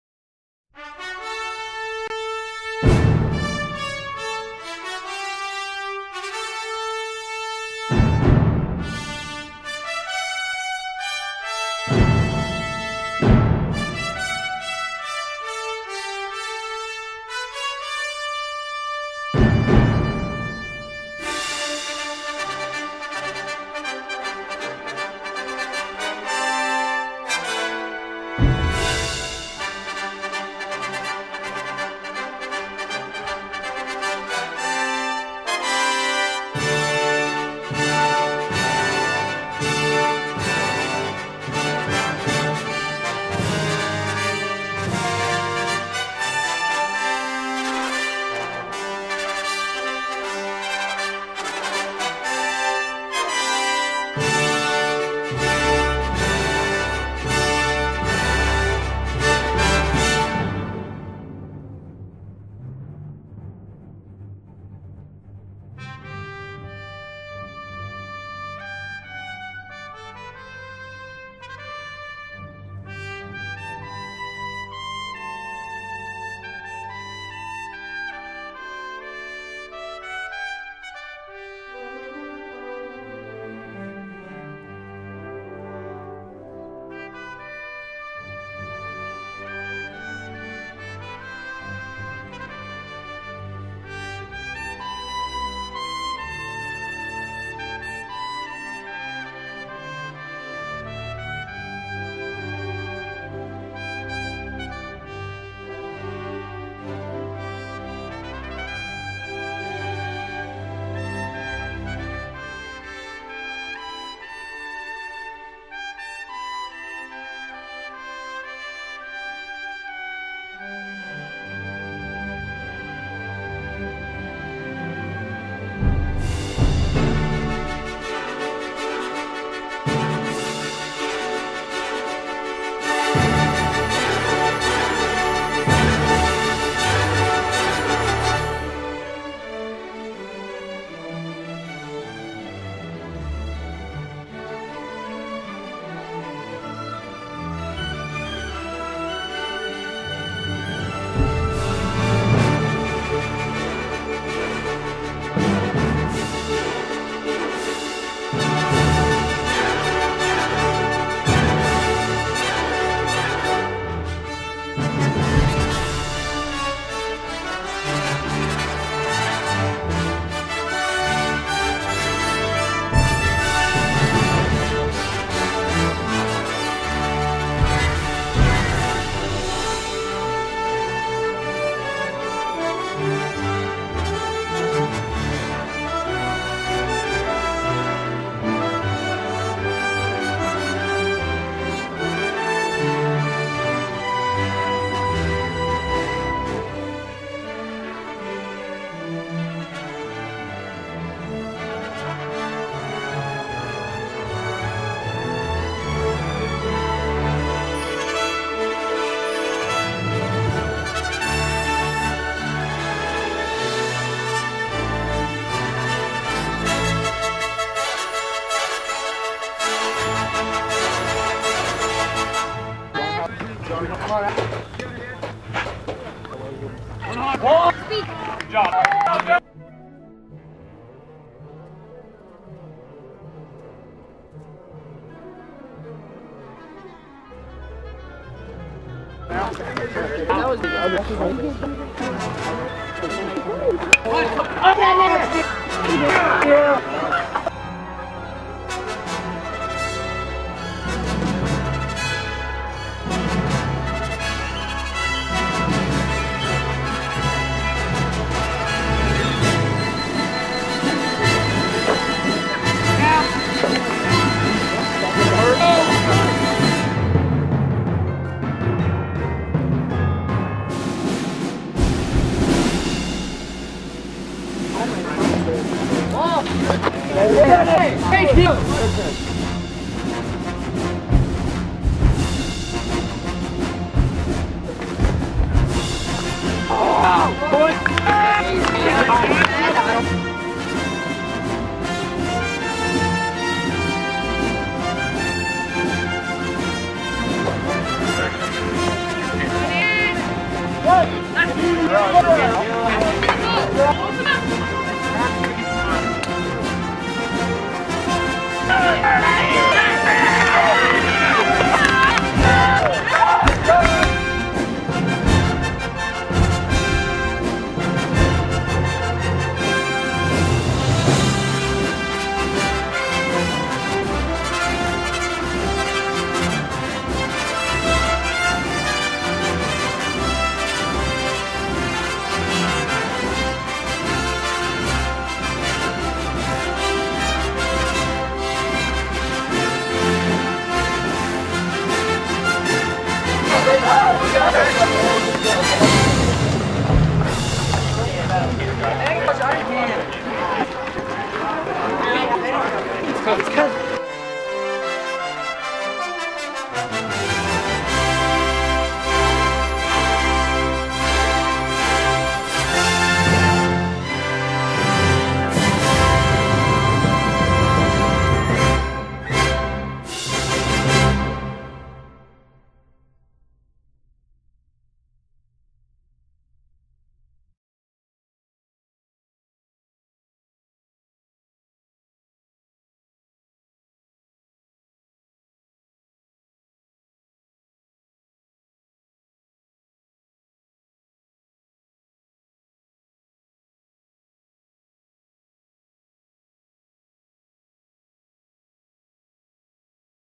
Canal Winchester 8th Grade Boys Baseball vs. Amanda Clearcreek Middle School
Great game !  As well as final game of the year !  In spite of the ending, it was a terrific game to watch which ran very late.  The game was tied at the end of 7 and went into extra innings.  The last inning of play (9th) was to be the LAST regardless of outcome of the score due to darkness.  CW Indians did not score in their final at bat and needed to hold Amanda Clearcreek to no runs to end the game in a tie.  Alas!  The gods were against us !